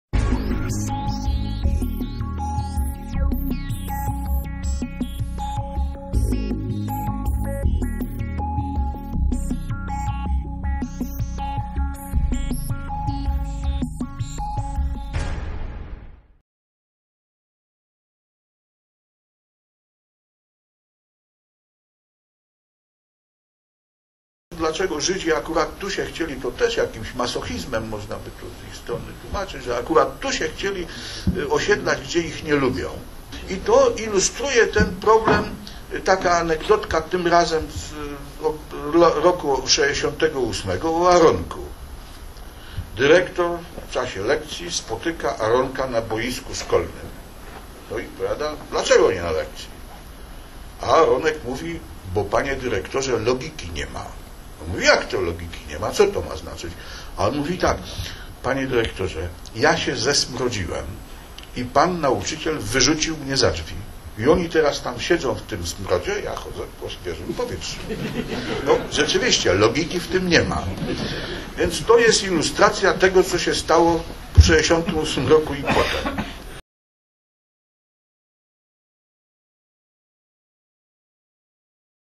Michalkiewicz opowiada dowcip o Aaronku czyli "Logiki nie ma"
Wypowiedź Stanisława Michalkiewicza podczas debaty z udziałem Leszka Żebrowskiego i Tadeusza Płużańskiego.
Spotkanie zorganizowało Stowarzyszenie Bełchatowianie dla Narodu - wrzesień 2017